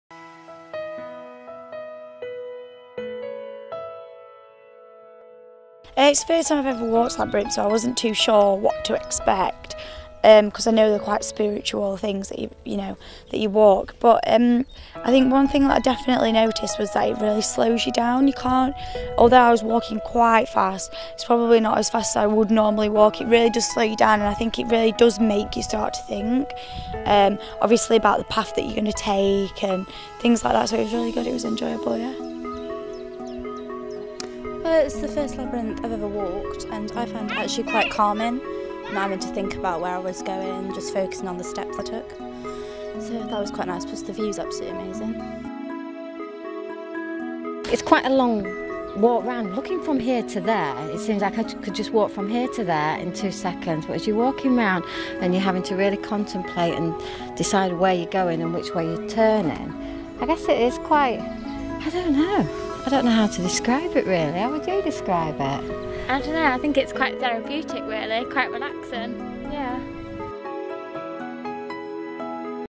World Labyrinth Day
Filmed on world labyrinth day on the 3rd May 2014, at the 12th century labyrinth in Alkborough, Scunthorpe.